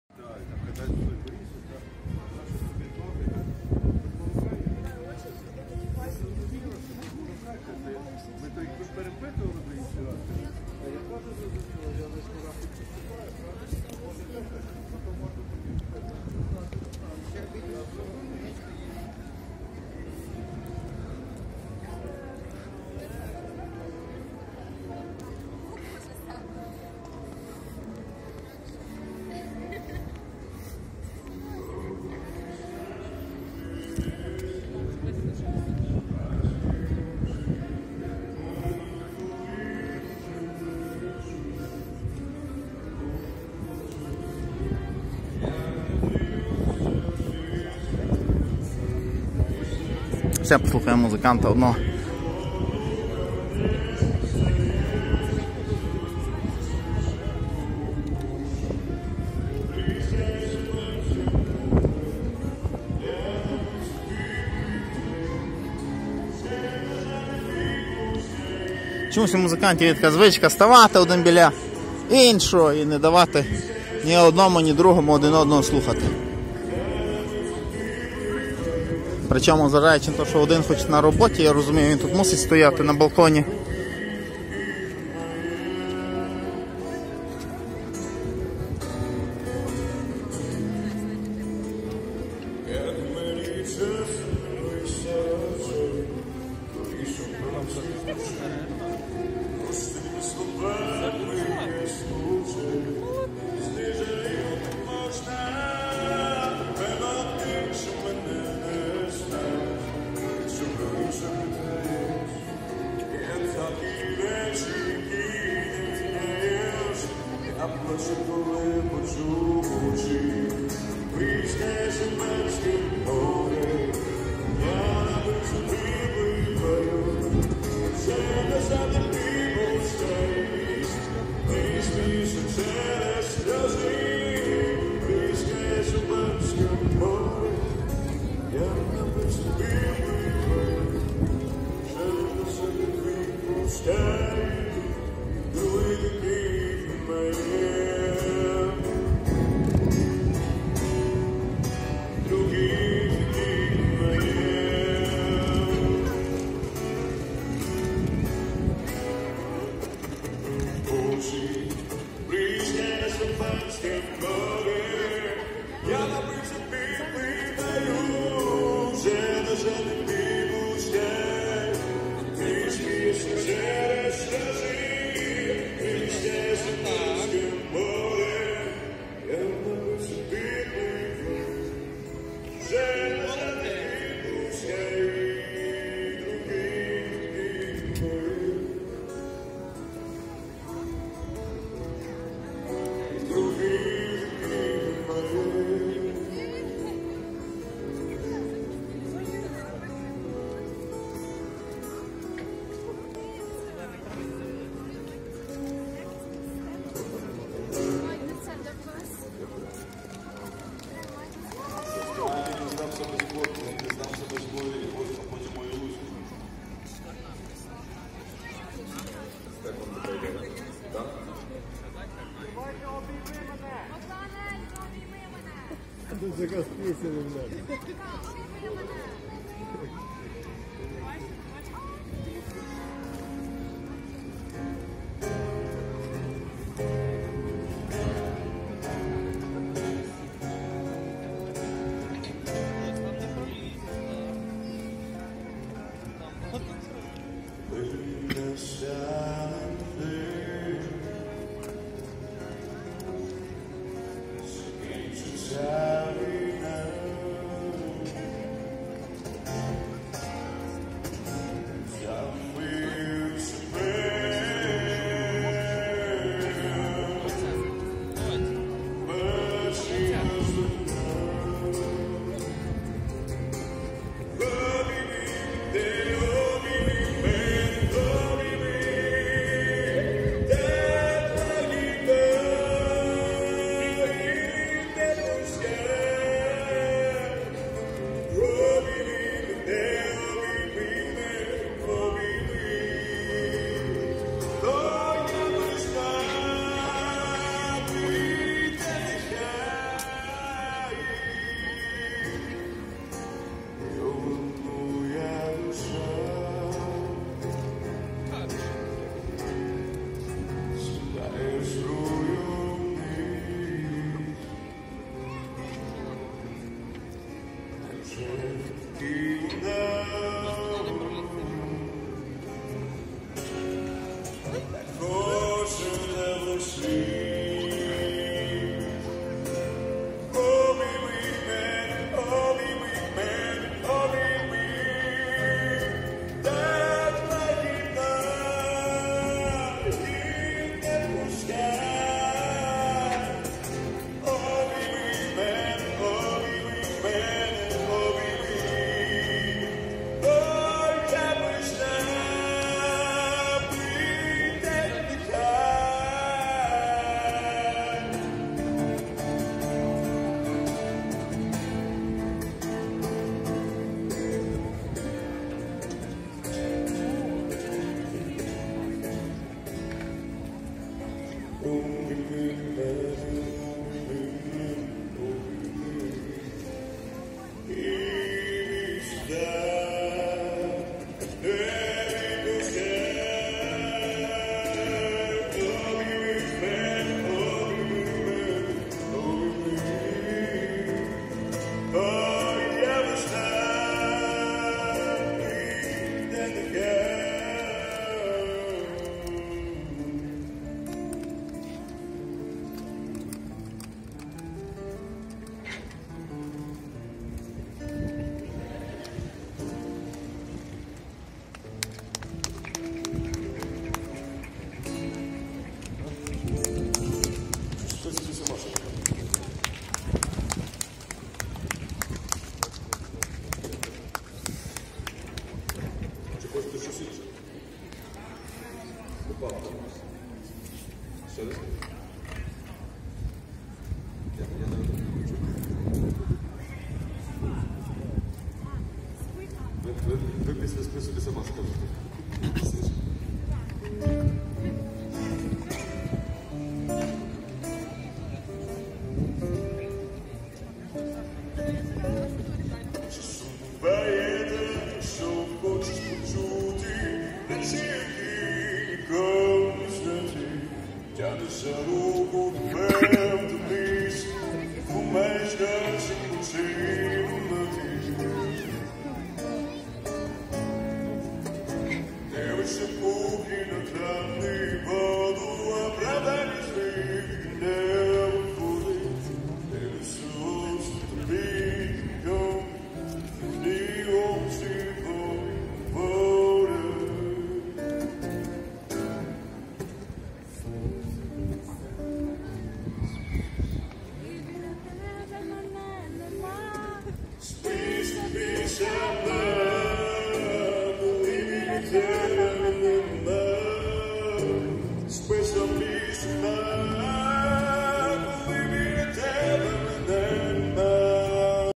Львів 2023: музика на балконі Площі Ринок нічне життя Lviv nightlife
Львів 2023: музика на балконі Площі Ринок нічне життя Lviv nightlife 24 березня 2023 р.